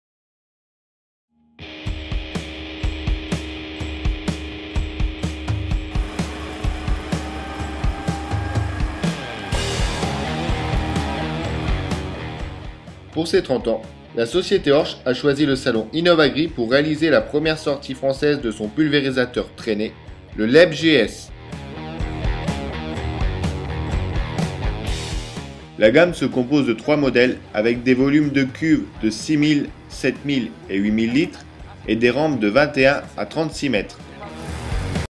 Pulvérisation : Première sortie française pour le Horsch Leeb GS
Pour ses 30 ans, Horsch présente pour la première fois le pulvérisateur Leeb GS en France. Ce pulvérisateur trainé, est muni d'un essieu suiveur. Il est disponible, avec des volumes de cuves 6000, 7000, et 8000 litres, ainsi que des rampes de 21 à 36m. Il reçoit le suivi de rampe Boom trac Pro, ainsi que le système de rinçage en continue CCS.